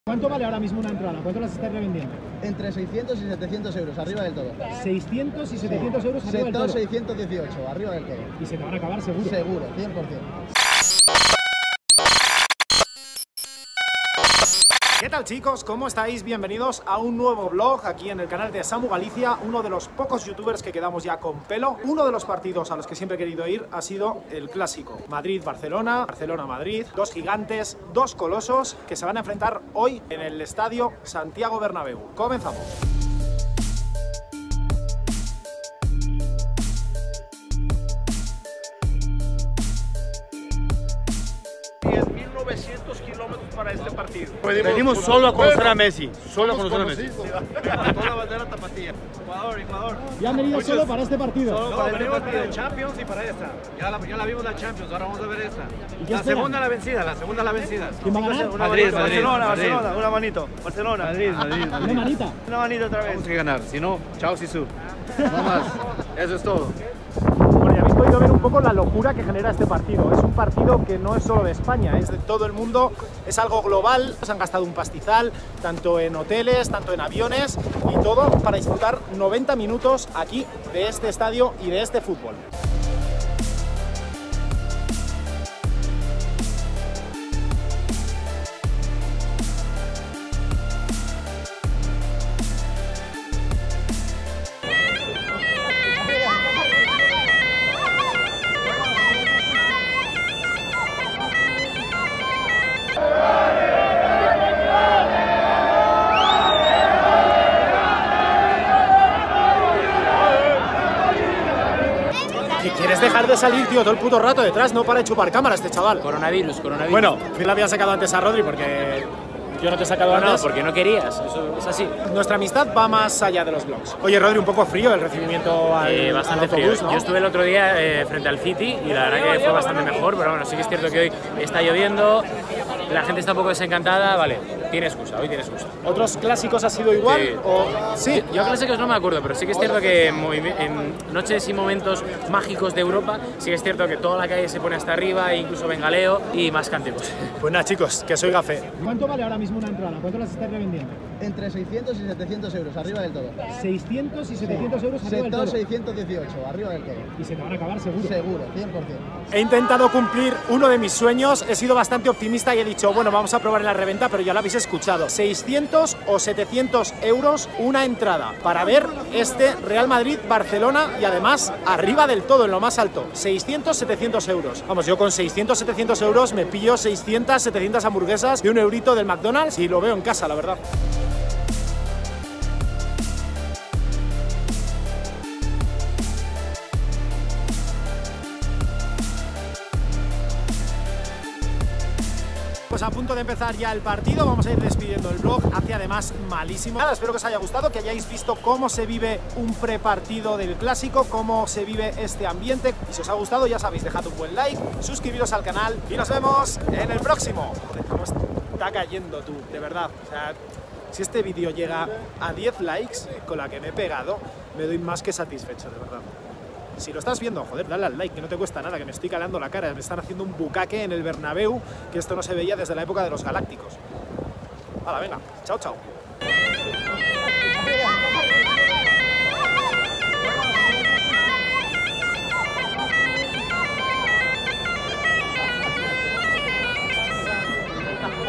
¿CUÁNTO CUESTA VER UN REAL MADRID - BARCELONA? ¡VIVIMOS EL CLÁSICO DEL FÚTBOL!
¡Sígueme, copón!